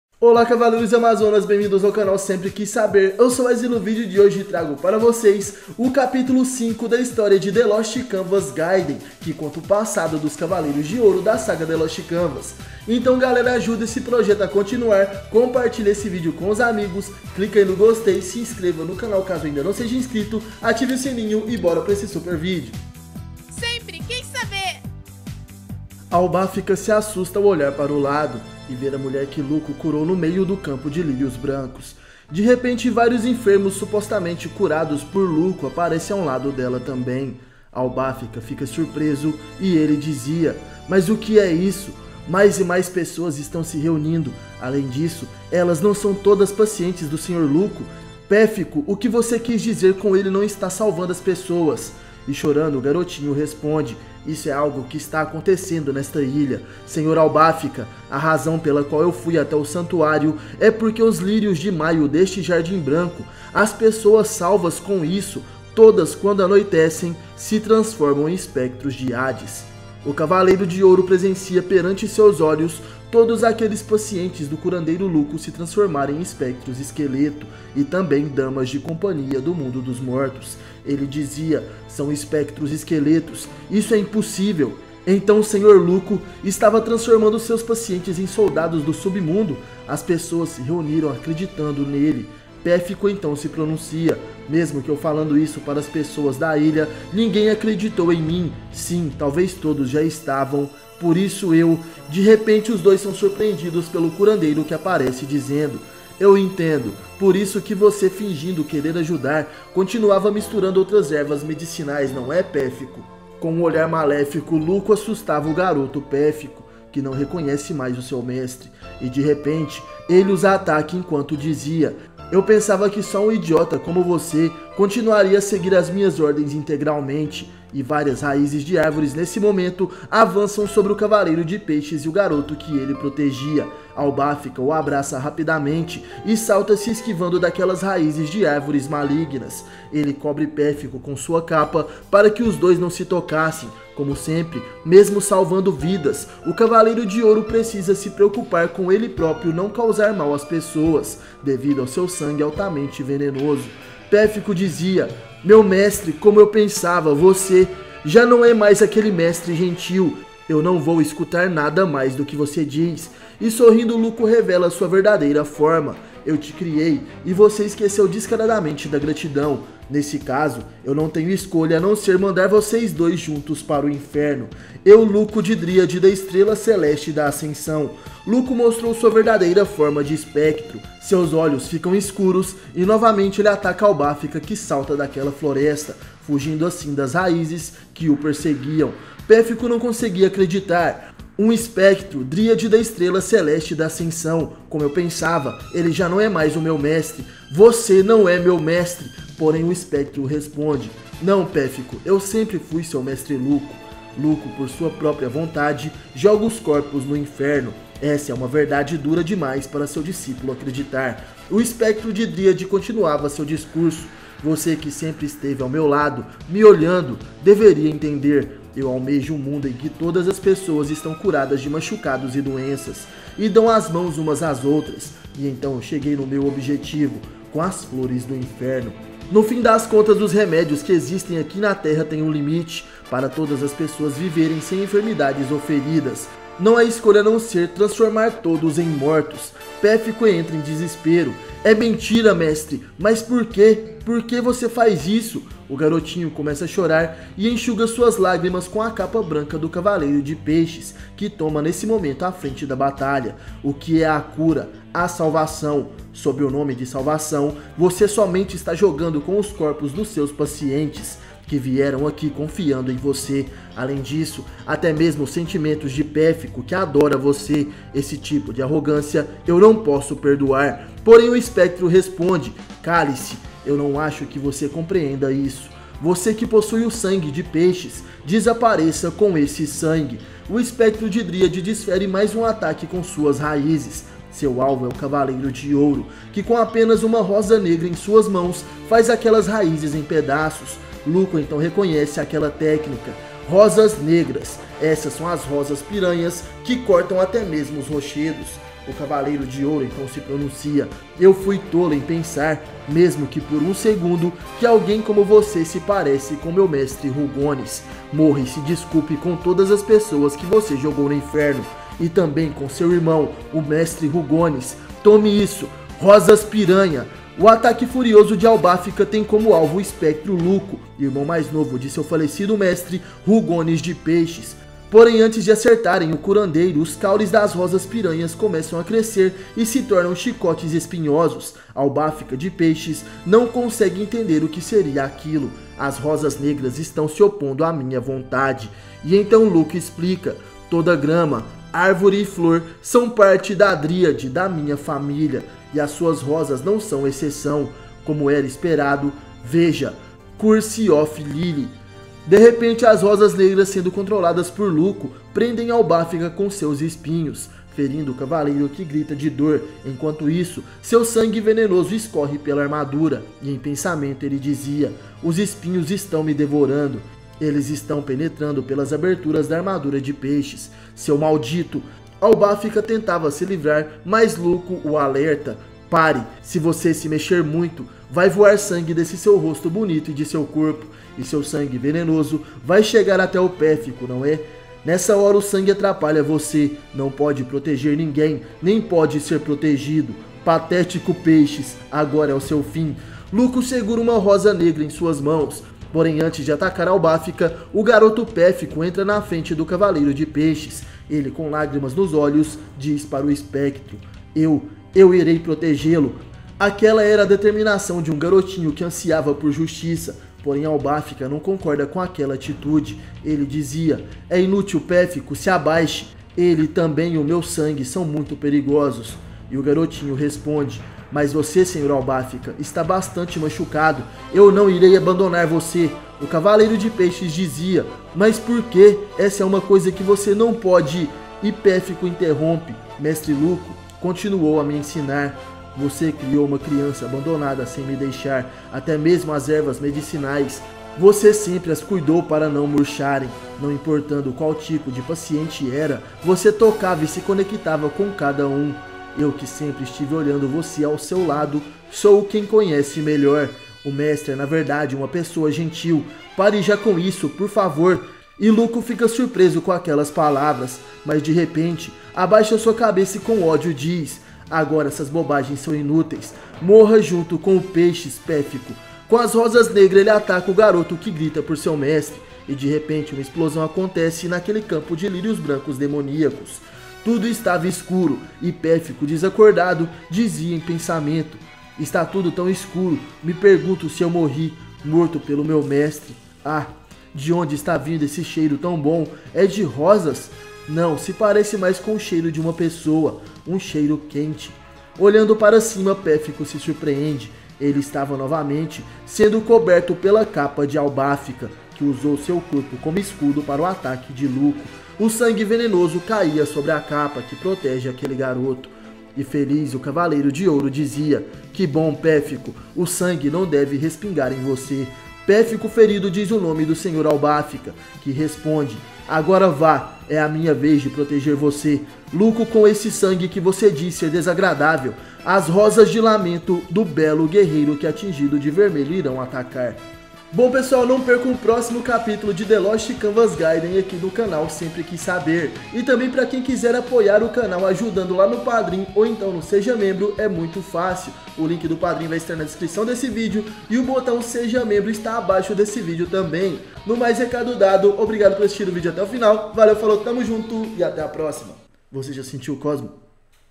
A Verdade sobre o Espectro Luco de Dríade! The Lost Canvas Gaiden - Capítulo 05 / Saint Seiya
Albáfica descobre a verdade sobre o Espectro Luco de Dríade!

Categoria do Vídeo: Review do Mangá Saint Seiya The Lost Canvas Gaiden.
O Mangá Saint Seiya The Lost Canvas Gaiden é escrito e desenhado pela autora Shiori Teshirogi.